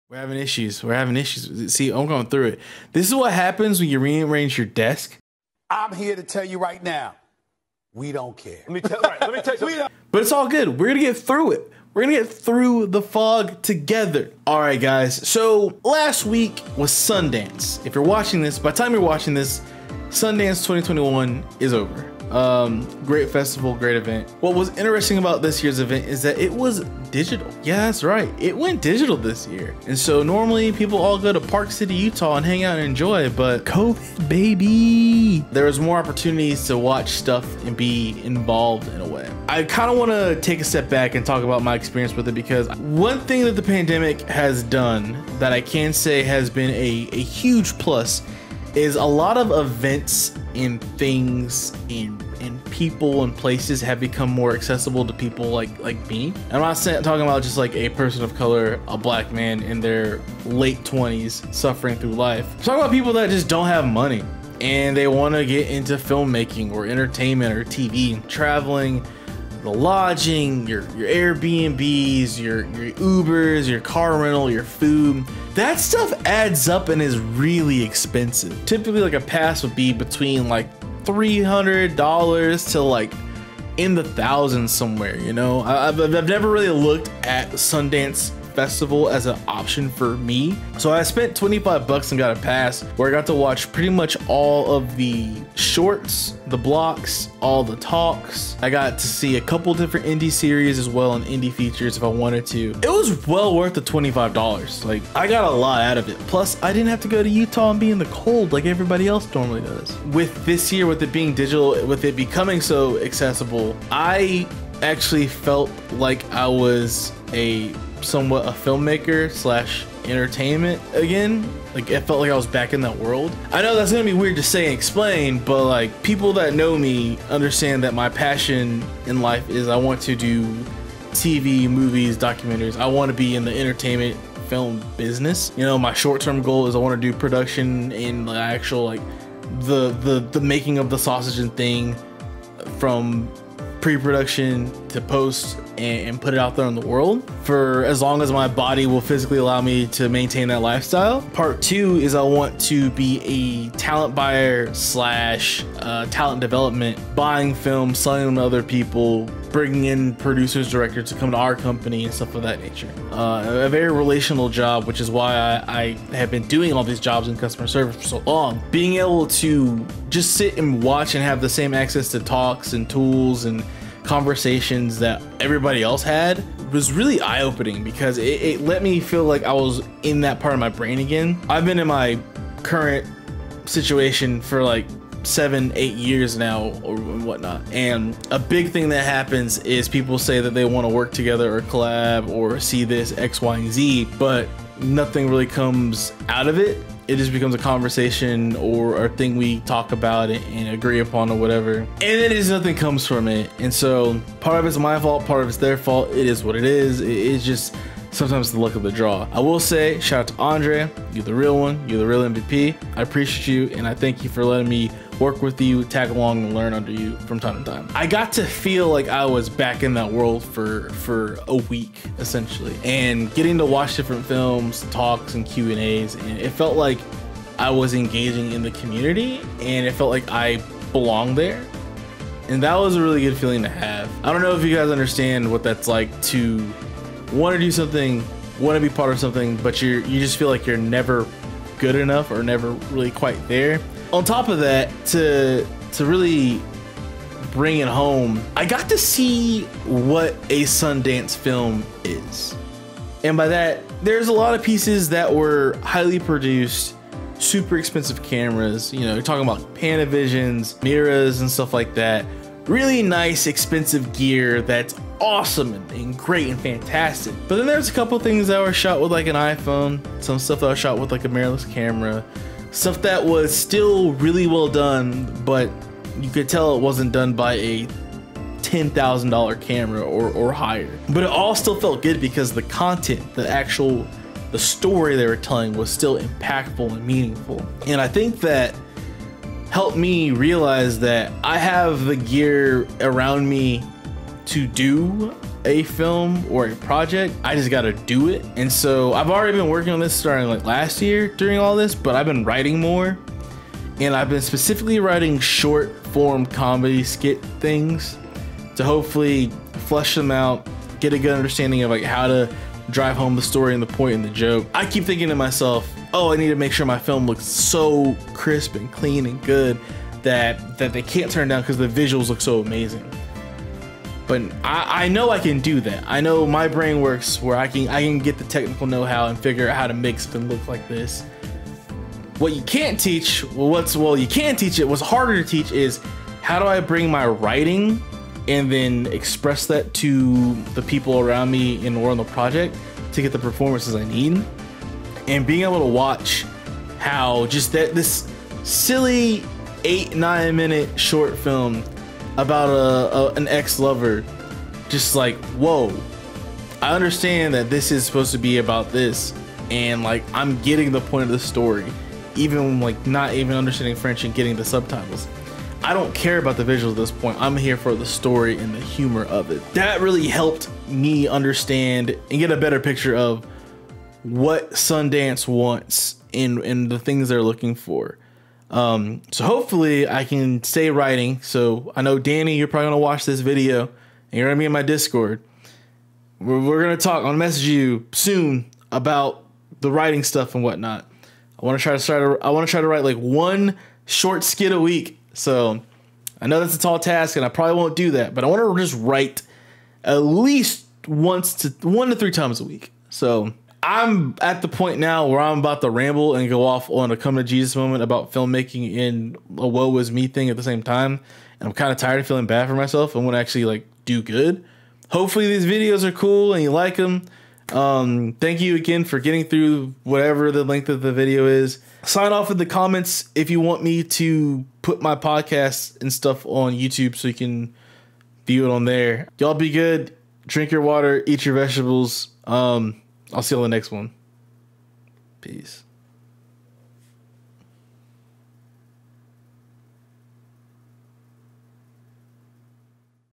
A Little Fogi I Episode 4: Fogi and the Sundance Festival
We're back with another grand episode of A Little Fogi. This time we talk about Sundance and my insecurities in the world of filmmaking.